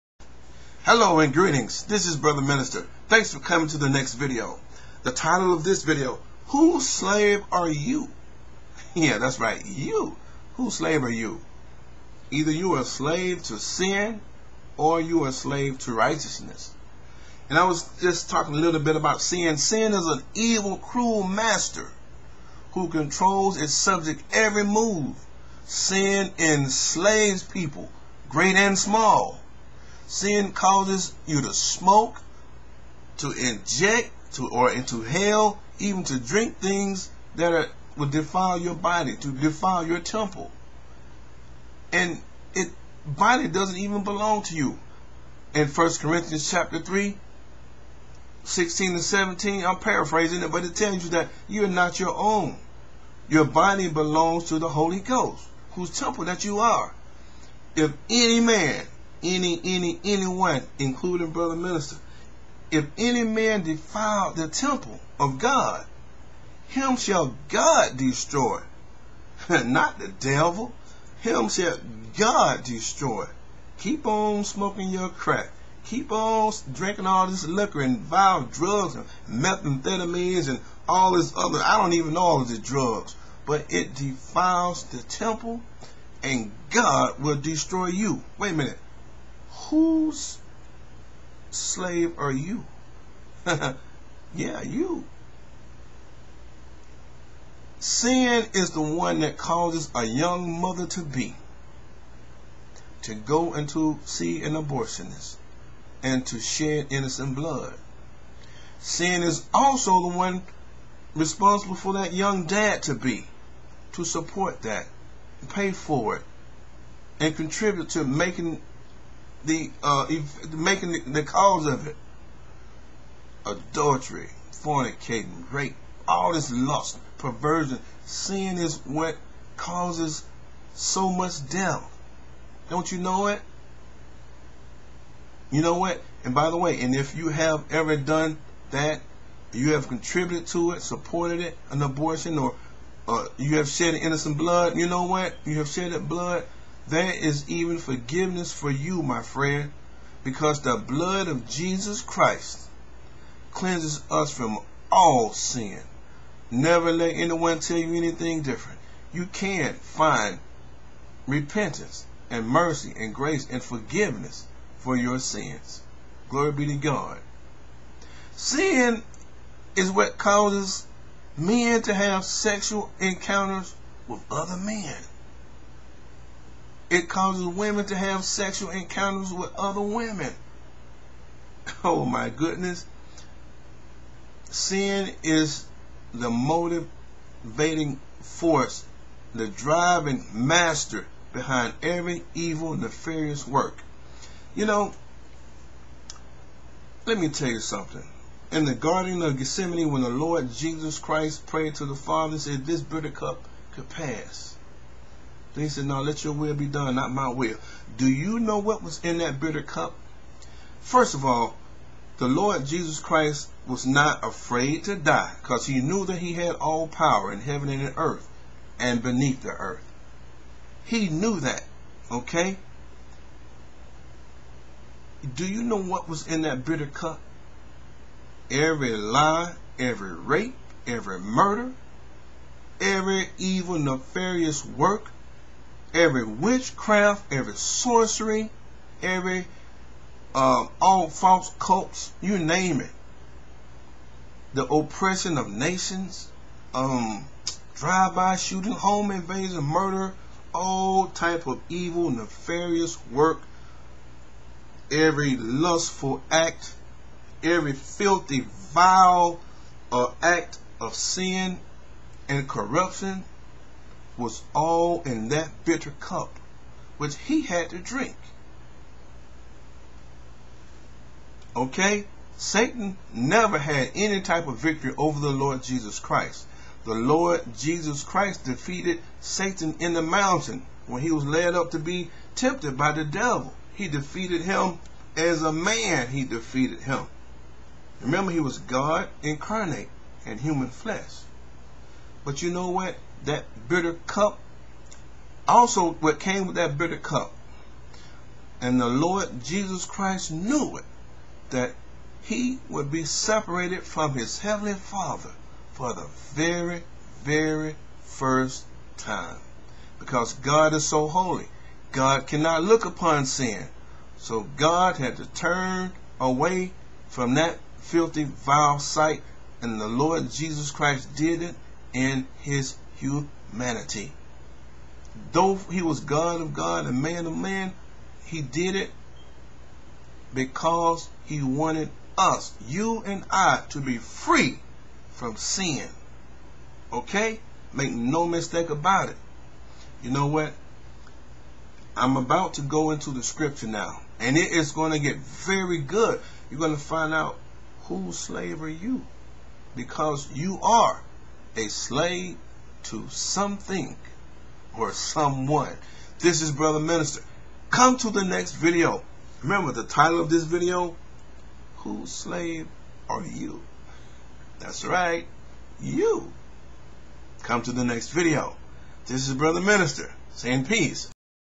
Whose SLAVE are YOU??? PT 2
You ARE a SLAVE. Whom do YOU belong to??? You are either a Slave to sin, or you are a Servant of Righteousness.
Romans Chapter 6